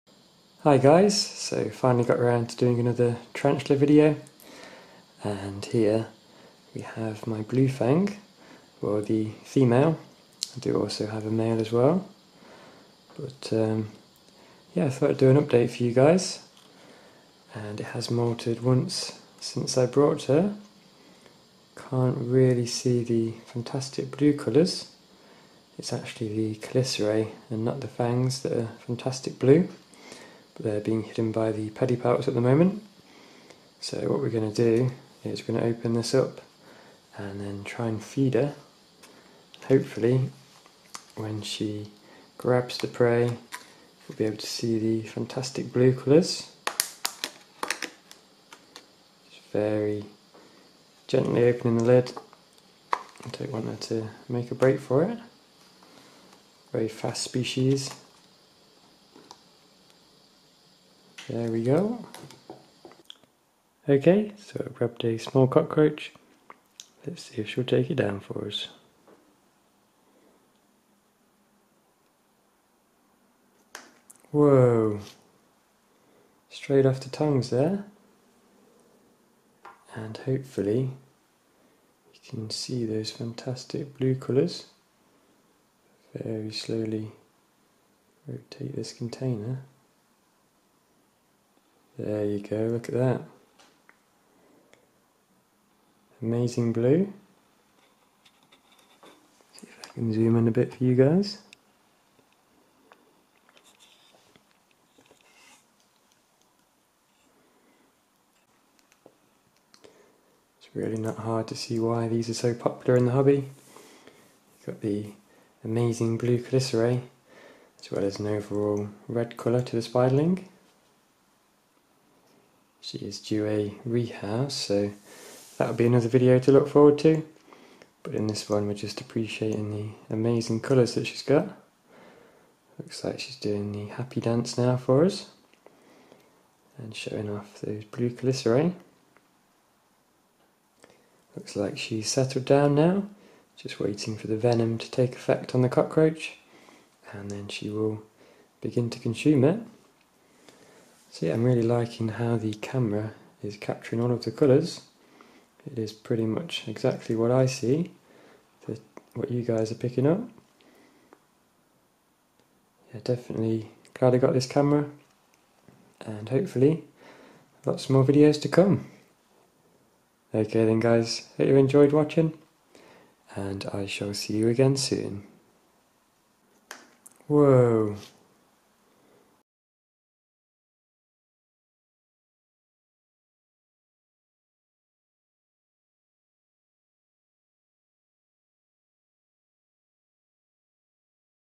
Blue Fang Tarantula! (Amazing Colours!!)
An update on my female blue fang (Ephebopus cyanognathus)